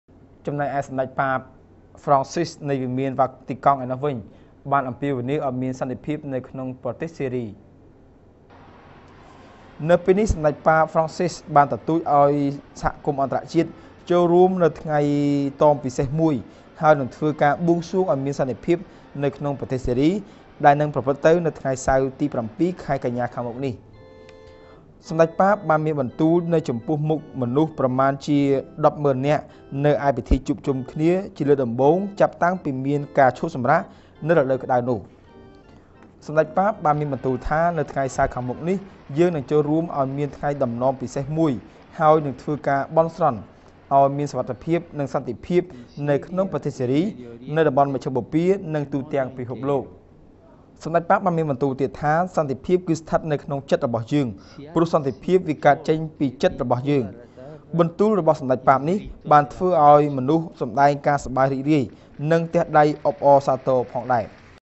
BTV Inter Papa 5-9-2013
សម្តេច PAPA FRANCISCO
 បាបអំនាវឲ្យមានសន្តិភាពនៅក្នុងប្រទេស SYRIA